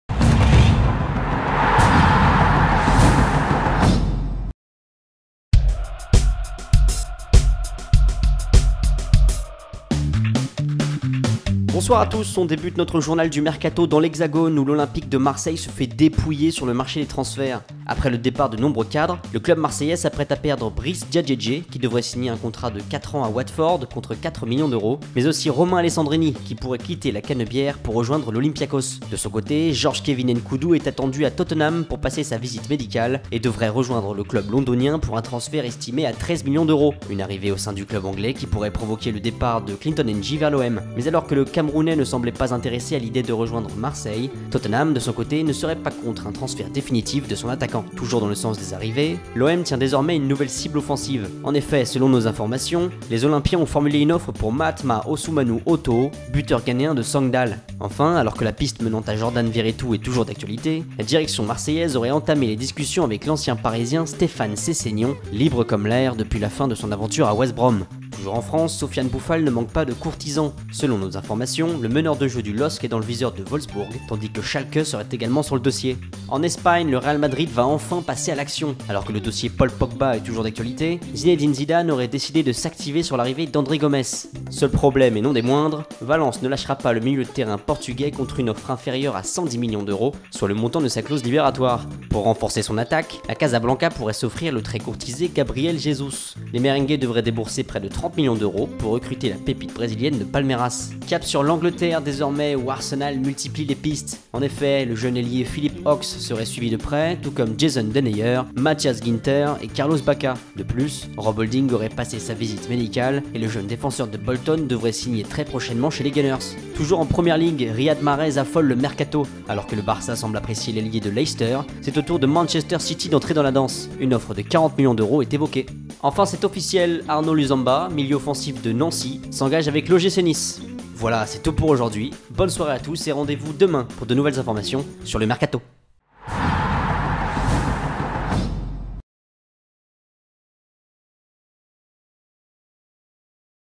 L'OM cible Njie, le Real Madrid fonce sur André Gomes et Gabriel Jesus (Journal du Mercato)
Au programme de ce Journal du Mercato : l'Olympique de Marseille multiplie les pistes et le Real Madrid de Zinedine Zidane qui passe à l'offensive.